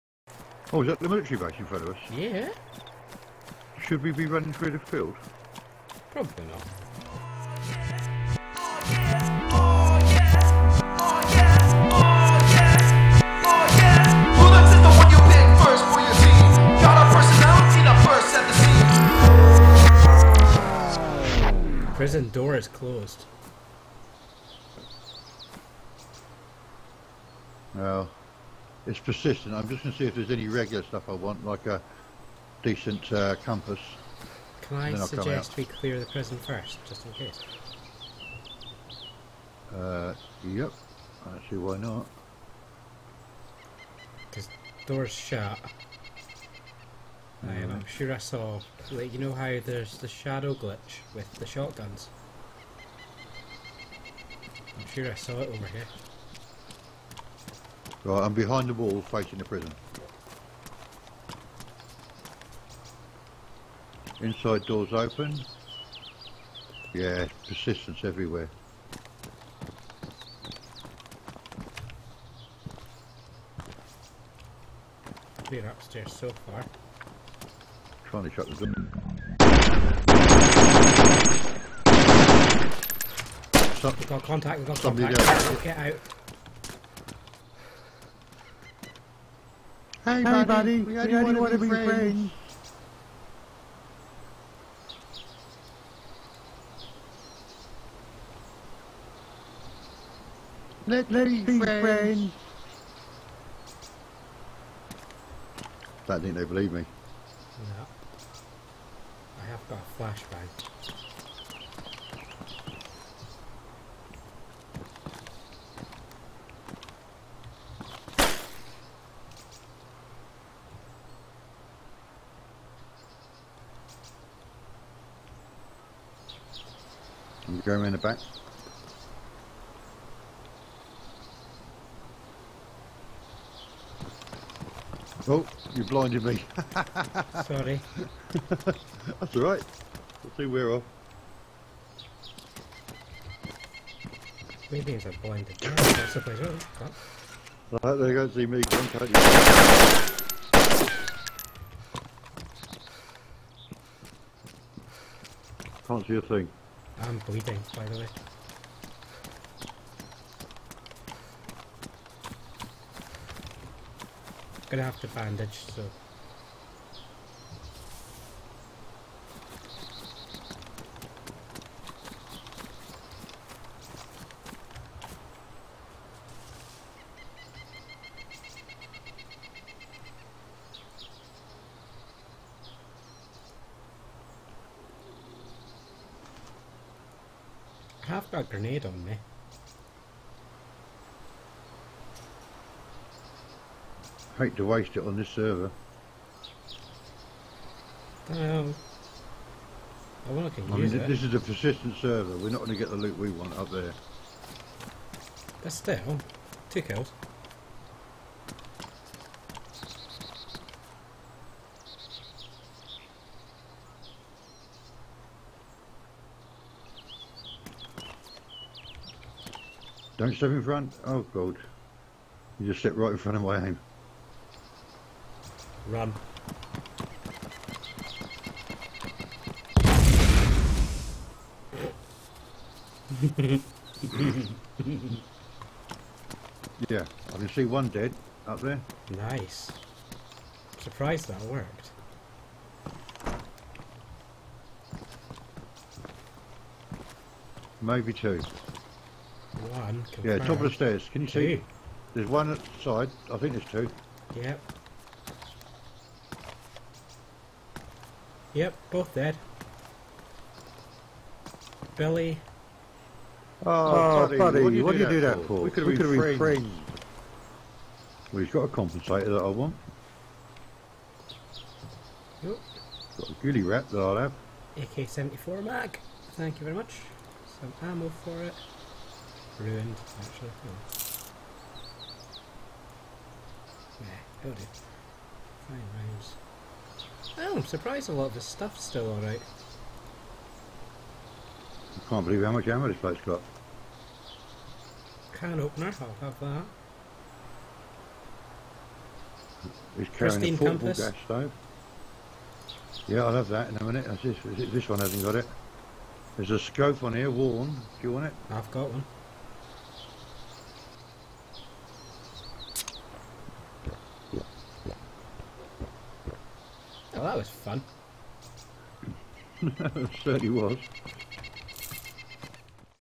Boom, Blast and Wrecked
Myself and Allan clearing the prison building at Vybor Military Base. Not quite "breach and clear" and more "lob in a frag and hope for the best"